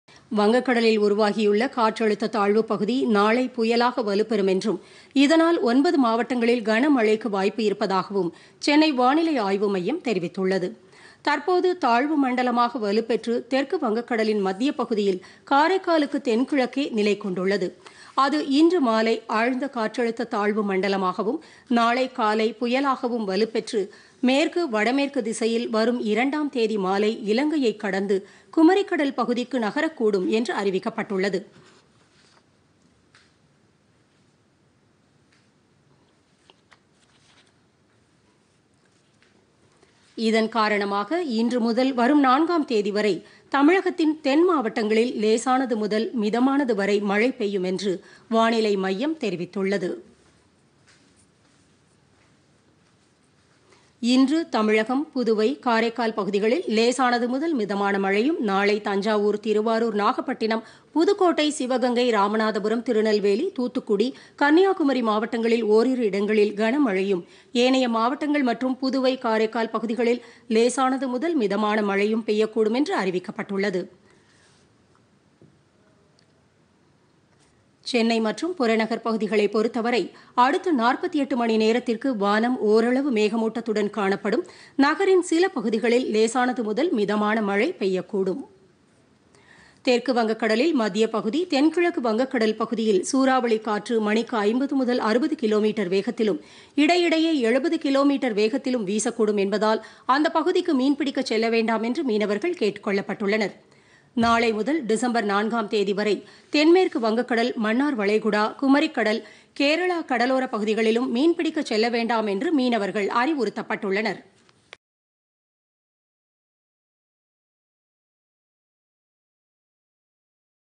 9 மாவட்டங்களில் கனமழைக்கு வாய்ப்பு
வங்கக்கடலில் உருவாகியுள்ள  காற்றழுத்த தாழ்வுப்பகுதி நாளை புயலாக வலுப்பெறும் - ஒன்பது மாவட்டங்களில் கனமழைக்கு வாய்ப்பு - வானிலை ஆய்வு மையம்.